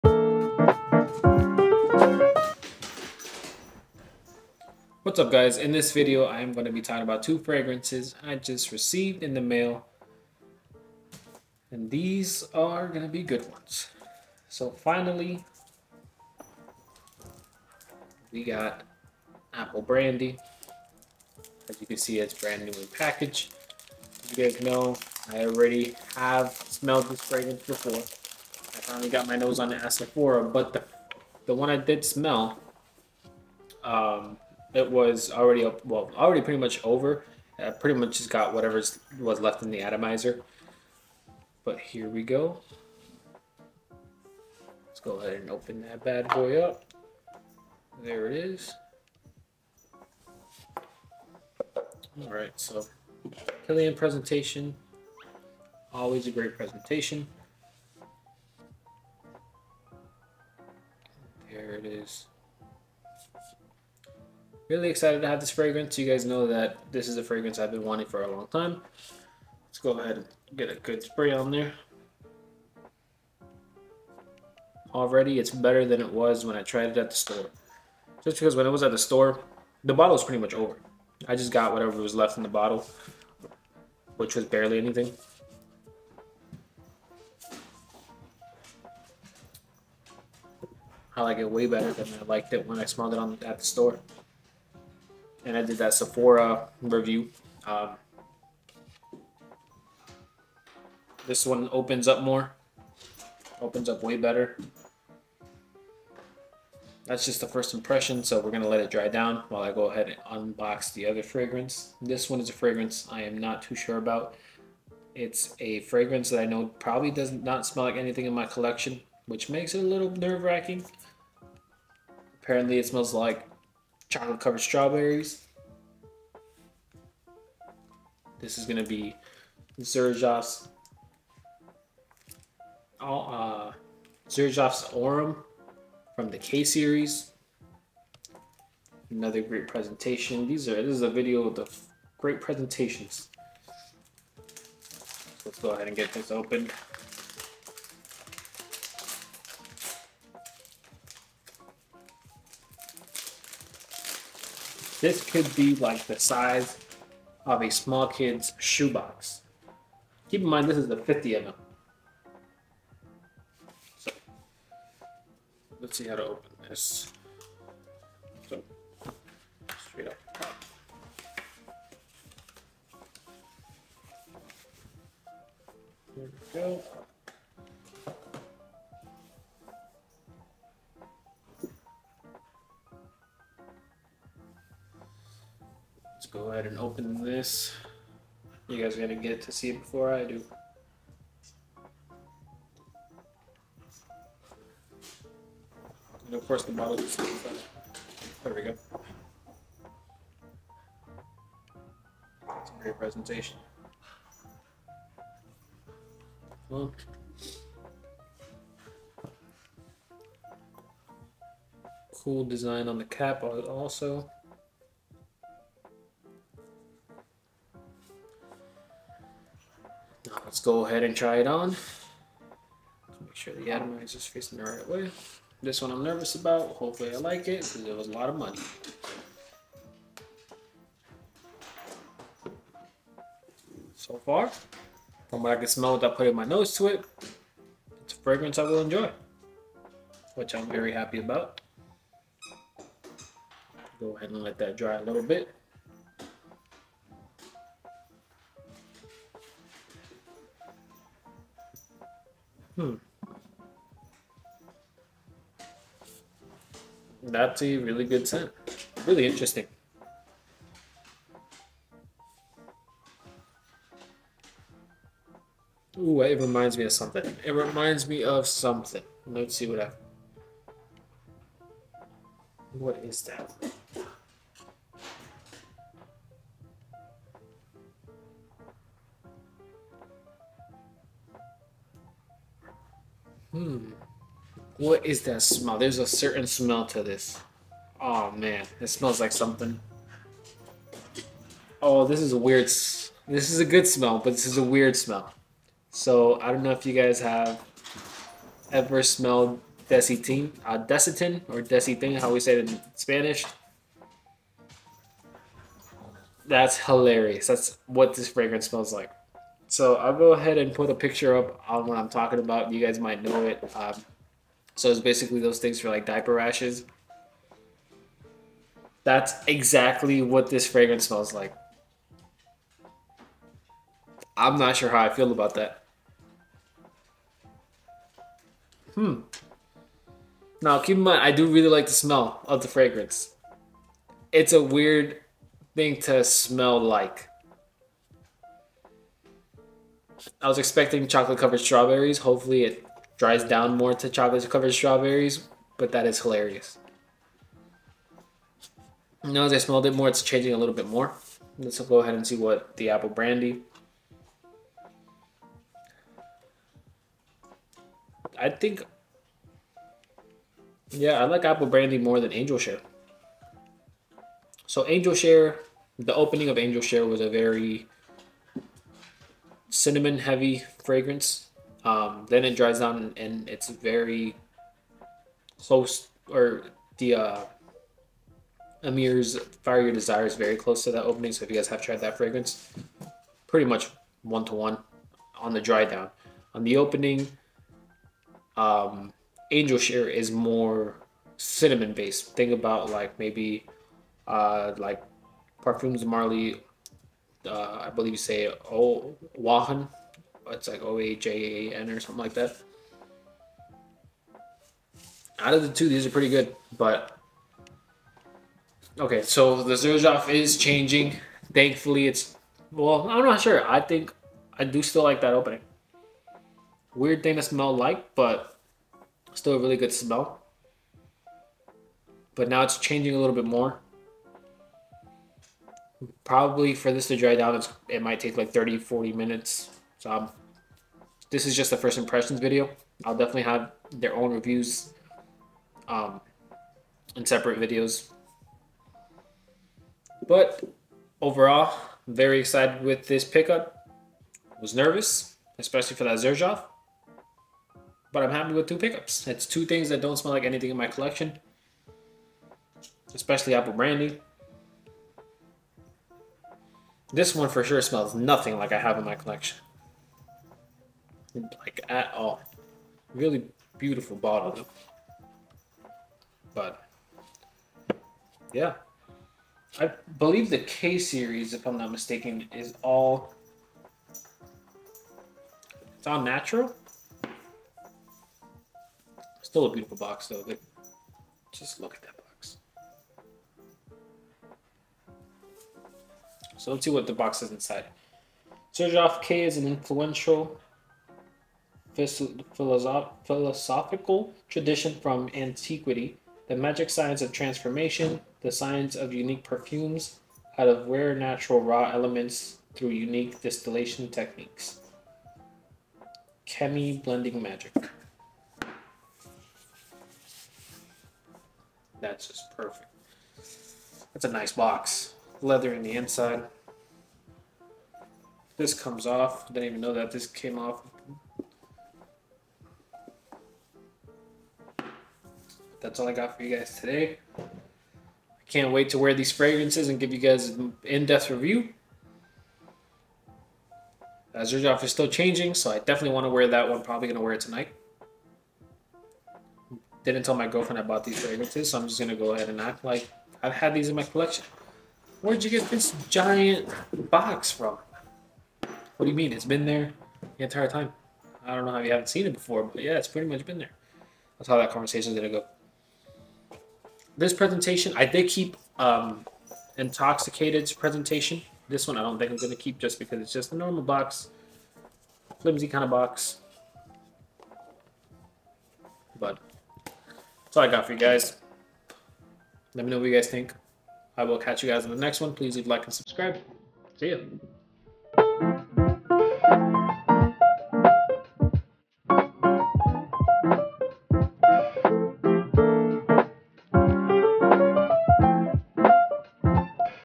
The Best Fragrance Duo - Xerjoff Aurum & Apple Brandy by Kilian
In this video, I give my first impressions on Apple Brandy and Xerjoff Aurum.